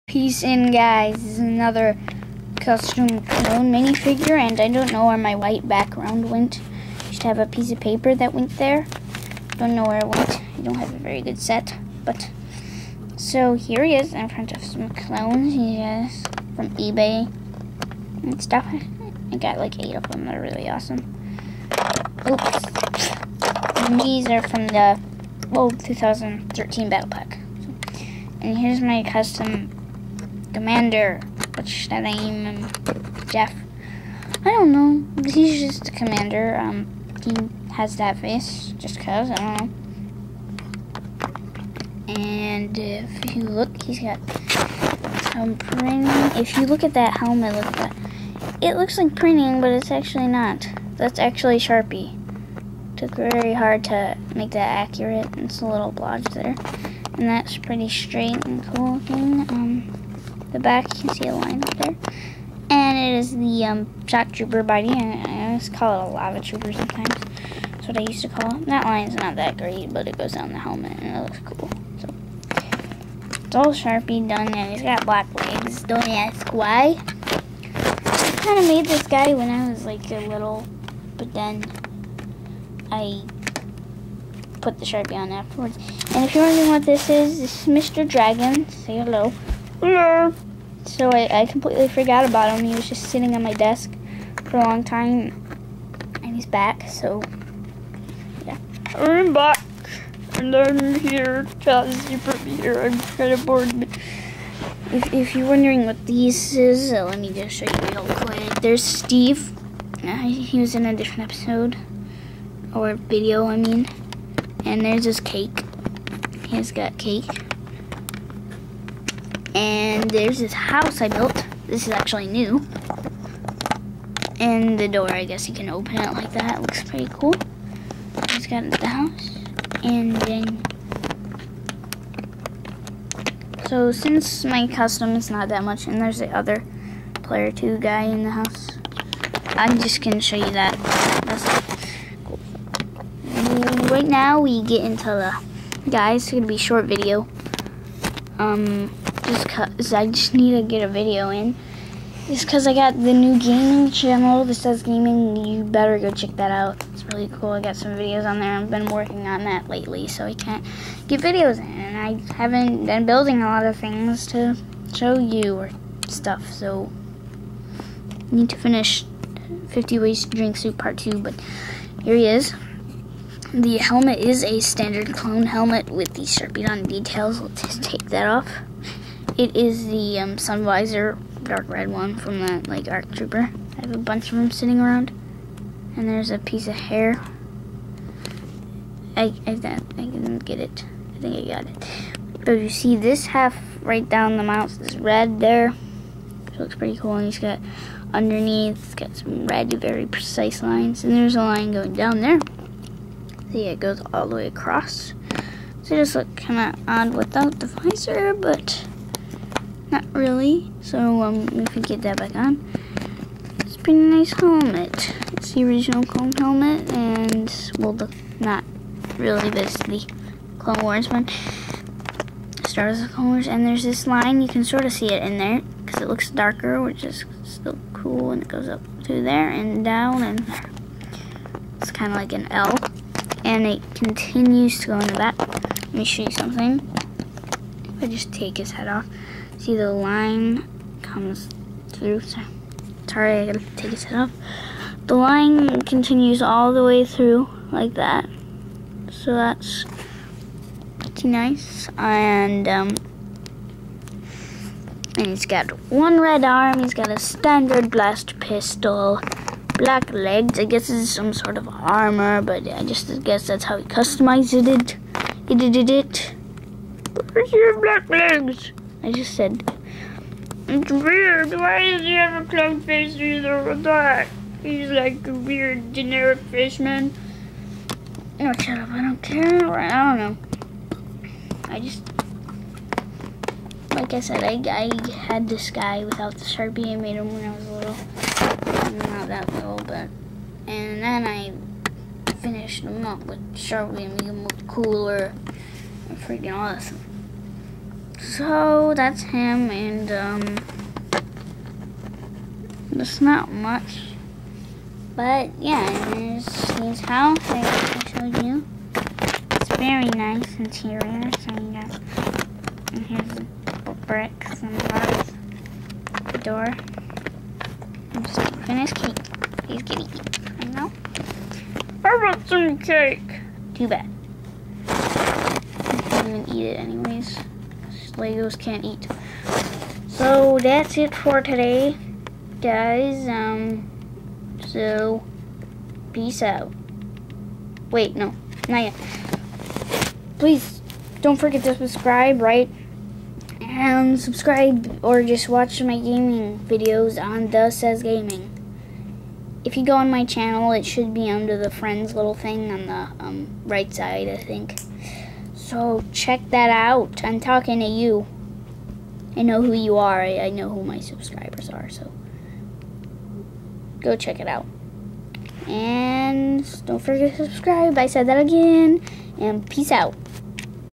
New LEGO custom clone commander
New LEGO custom clone commander made with Sharpie. I will also show you Steve's new house I built.